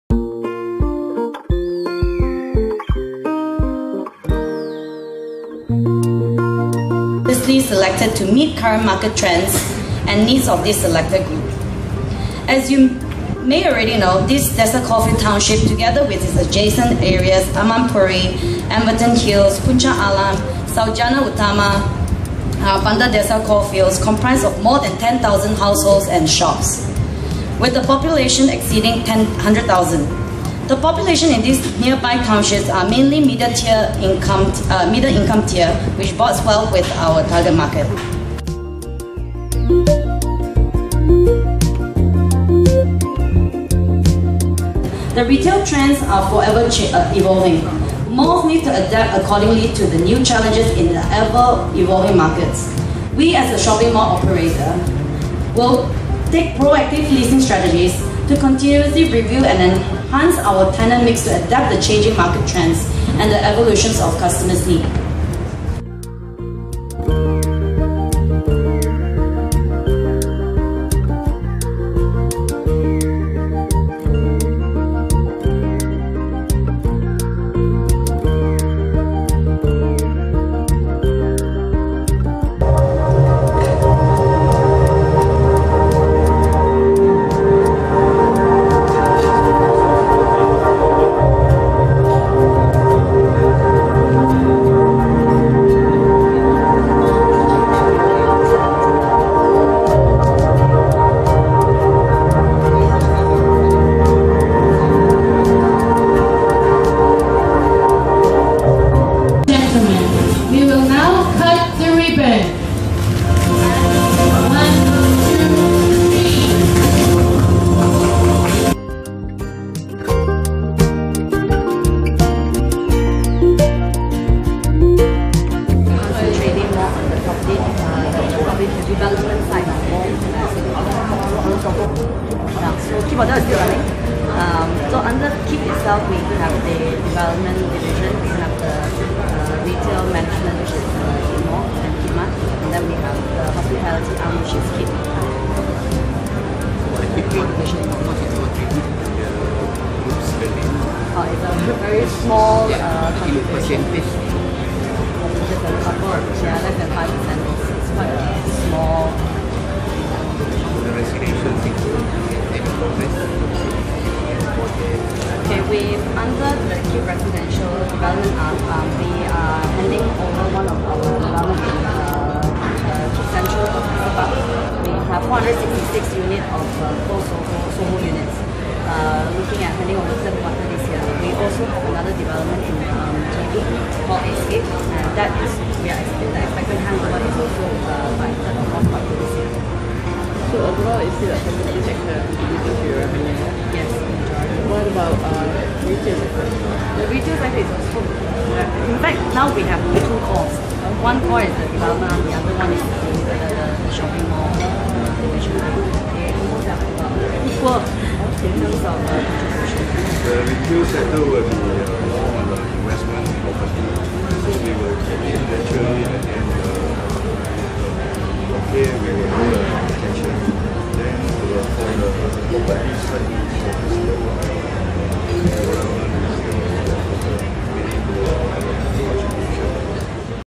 Majlis pecah tanah Kipmall Desa Coalfields
Majlis pecah tanah pembinaan pusat beli belah kesembilan Kip Group Companies iaitu KipMall di Desa Coalfields.

Pusat beli belah KipMall di Desa Coalfileds itu merupakan projek setinggi dua tingkat setengah dengan GDC sebanyak RM50 juta itu dibina di atas tapak seluas tiga hektar.

Ia mempunyai keluasan lantai kasar sebanyak 244,155 kaki persegi dengan ruang sewaan berjumlah 141,869 kaki persegi dengan turut menyediakan 436 ruang parkir kereta dan 240 parkir motosikal.